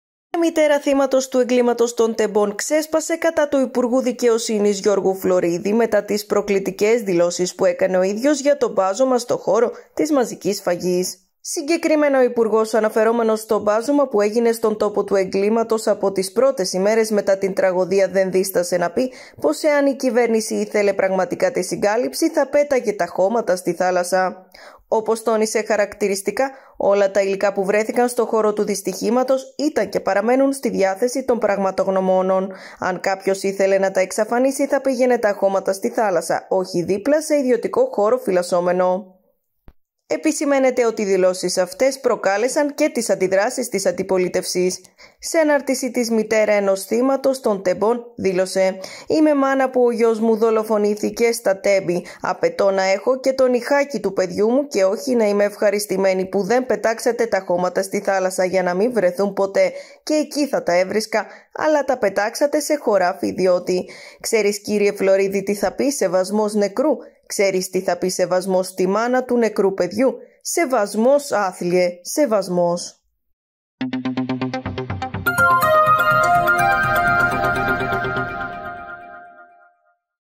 Μητέρα θύματος του εγκλήματος των Τεμπών κατά Γ.Φλωρίδη: «Ξέρεις τι θα πει σεβασμός νεκρού;»
Μητέρα θύματος του εγκλήματος των Τεμπών κατά Γ.Φλωρίδη: «Ξέρεις τι θα πει σεβασμός νεκρού;» | Pronews TV